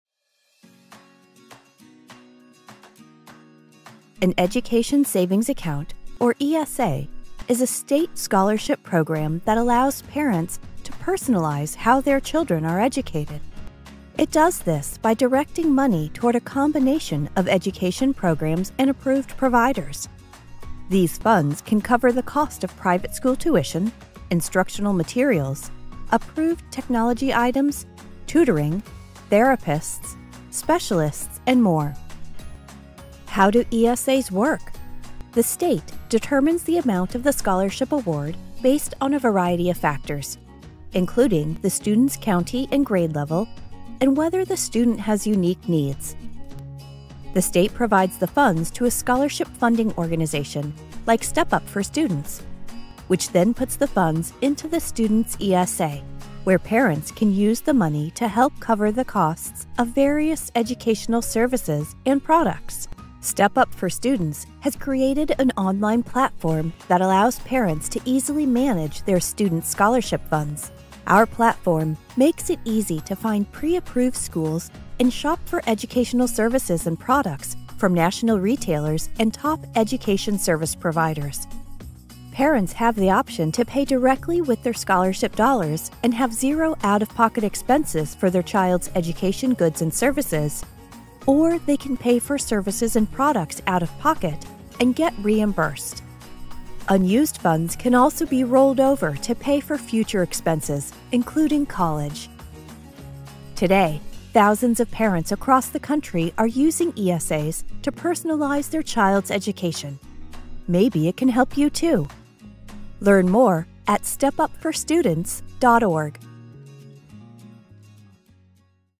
What is an ESA?
Welcome McKay Scholarship Families! What is an ESA? A different kind of scholarship- Educational Savings Accounts, or ESAs, are different than other state scholarships in that they allow parents to personalize the education of their children by directing money toward a combination of programs and approved providers. These include schools, therapists, specialists, curriculum, technology – even a college savings account. Watch the video to learn more!